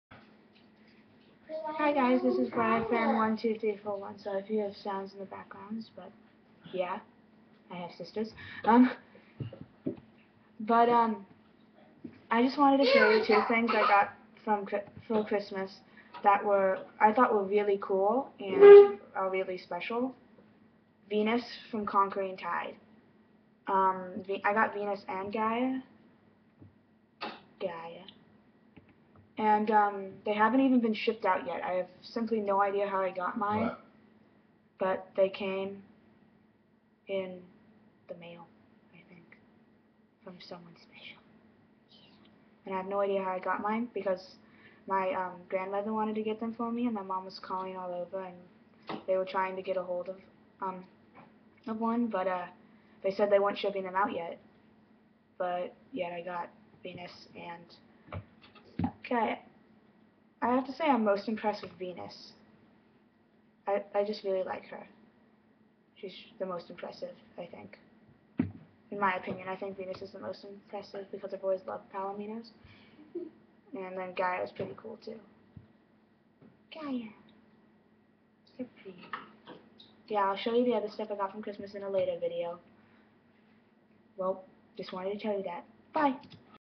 I GOT CONQUERING TIDE MODLE HORSES!!!!!!!!!
Okay i just really wanted to show you peeps. But I'm a bite confused because they have not even been shiped out yet they where going to be shiped out like three days after new years? yeah so i hope you all had a great Christmas and have a happy new year!